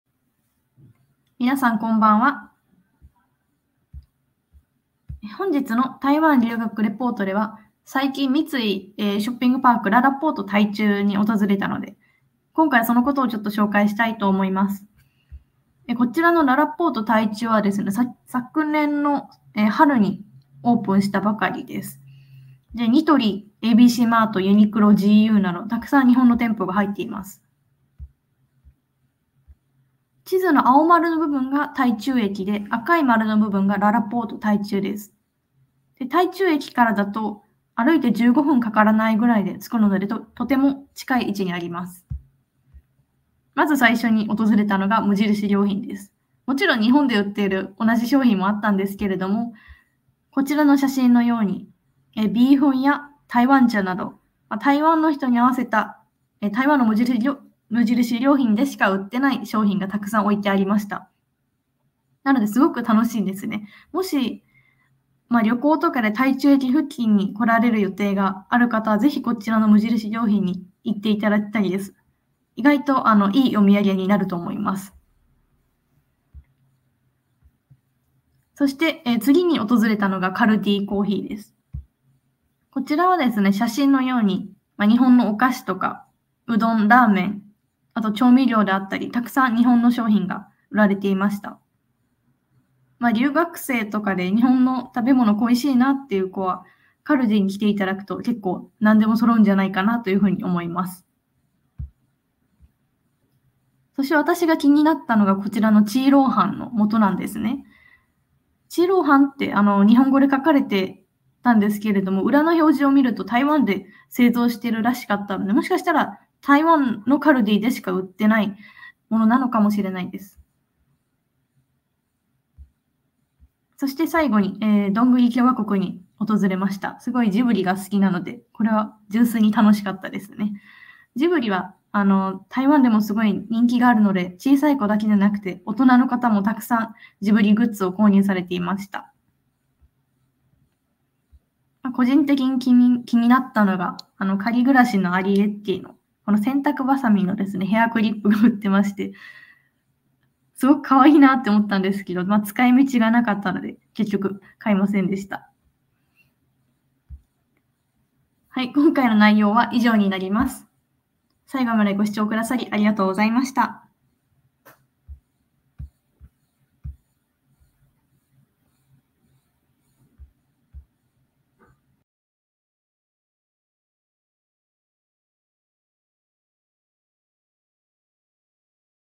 【すたんぷの台湾留学レポート】ららぽーと台中
皆さんこんにちは、そしてこんばんは。じゃっばーぼぇです！
いつもご視聴くださりありがとうございます。

昨年の春、台中に三井ショッピングパーク ららぽーと台中がオープンしました。この前、初めて訪れたのでちょっとだけ紹介したいと思います。